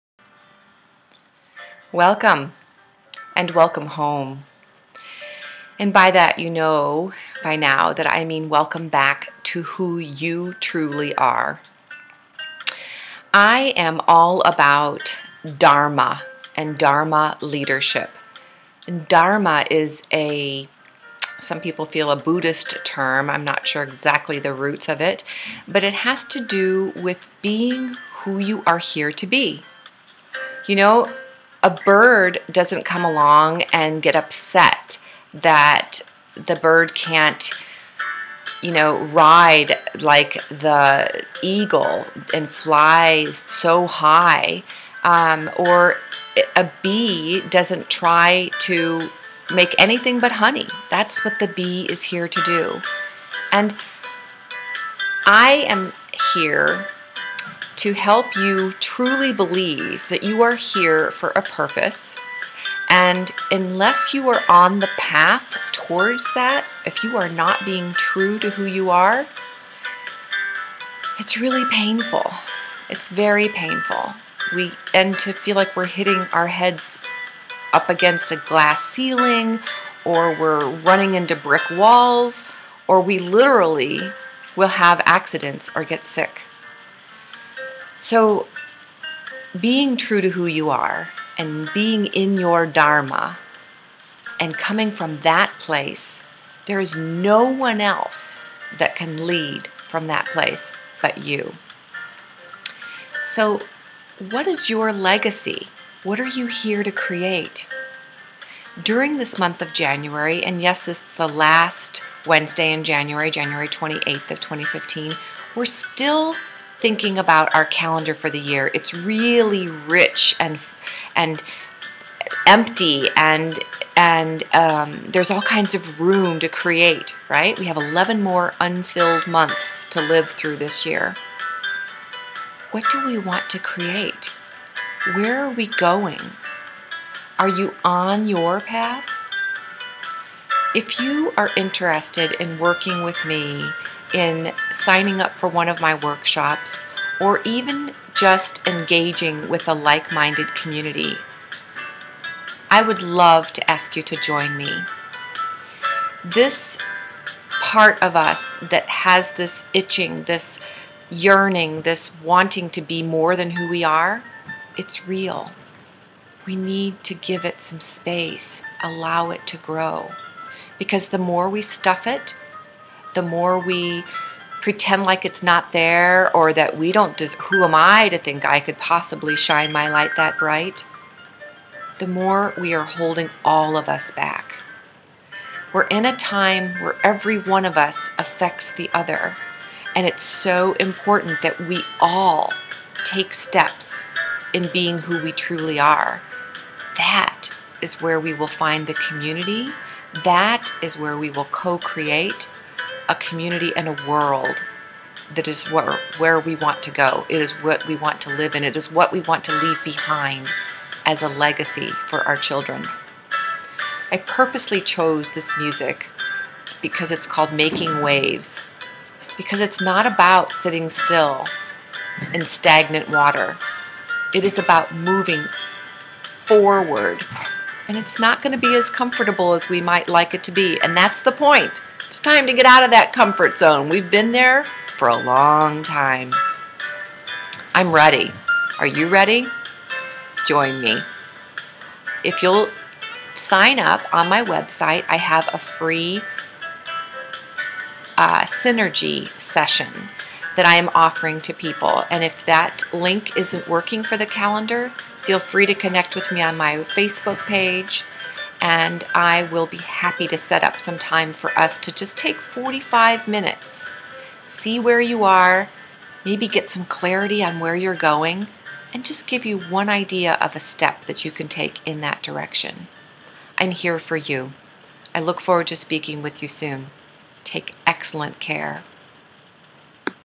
Wednesday Wisdom with Deborah LeeAnn - Dharma as Leadership
Are you living the highest and best version of your life? What are you here to do? To create? To leave behind as your legacy?